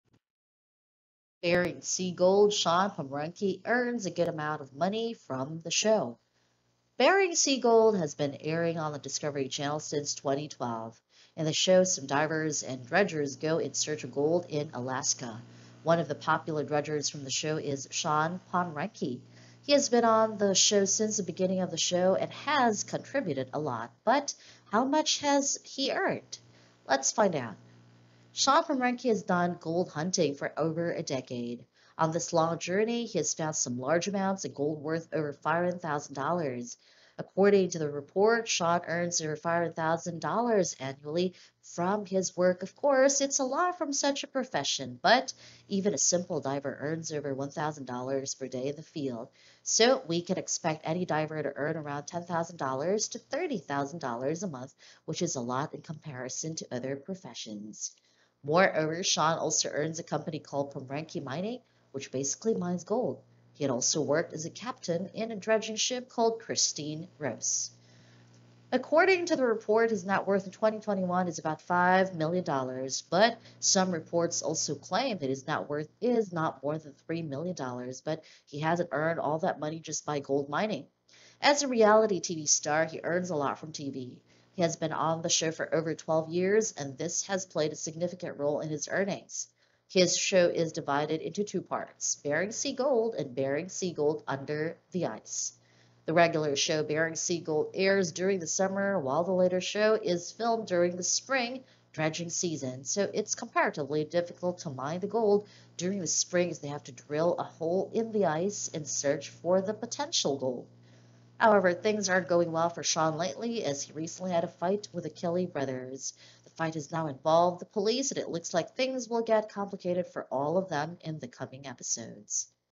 Bering Sea Gold: Shawn Pomrenke's Net Worth After Nearly 12 Years of Diving for Gold
Shawn Pomrenke is one of the oldest cast members of Bering Sea Gold. He has mined a massive amount of gold until now. But how much has he earned for himself? Does his job pay a good amount of money? Check out below for the answers.